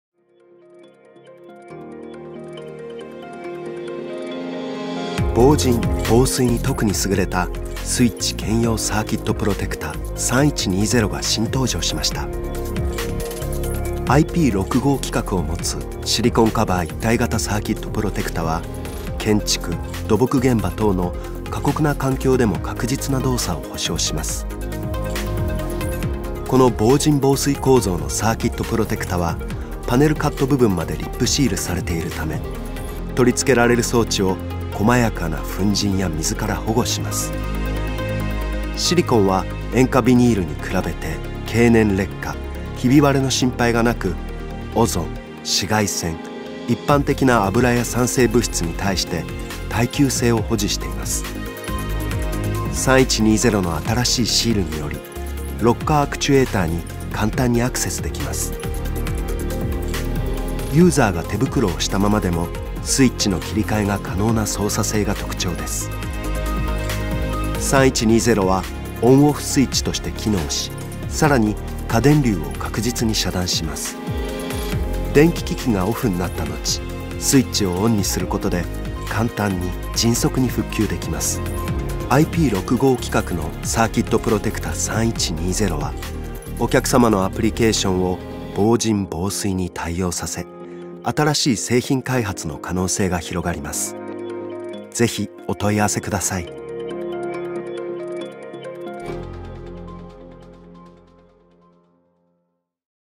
スイッチ兼用サーキットプロテクタ vol.3タフな環境でも使える IP65
防塵、防水に特に優れたスイッチ兼用サーキットプロテクタ3120が新登場しました
保護等級IP65（ろくごー）のシリコンカバー一体型サーキットプロテクタは、建築、土木現場等の過酷な環境でも確実な動作を保証します。
"この防塵防水構造のサーキットプロテクタは、パネルカット部分までリップシールされている為、取り付けられる装置を、細かな（こまかな）粉塵や水から保護します。
シリコンは、塩化ビニールに比べて経年劣化、ひび割れの心配がなく、オゾン、紫外線、一般的な油や酸性物質に対して耐久性を保持しています。
ユーザーが手袋をしたままでも、スイッチの切り替えが可能な操作性が特徴です。
"3120はON / OFFスイッチとして機能し、さらに過電流を確実に遮断します。

電気機器がOFFになった後、スイッチをONにすることで、簡単に迅速に復旧できます。
"IP65規格のサーキットプロテクタ3120は、お客様のアプリケーションを防塵防水に対応させ、新しい製品開発の可能性を広げます。